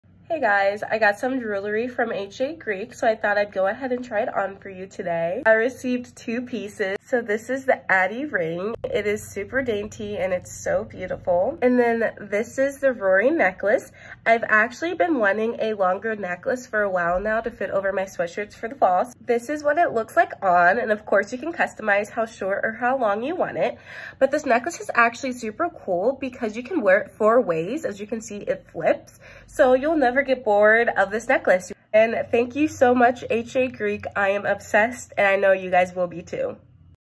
Perfect Greek jewelry for fall 🍂
Whether you're looking for cute, dainty rings or jewelry that can be worn 4 ways - we've got you!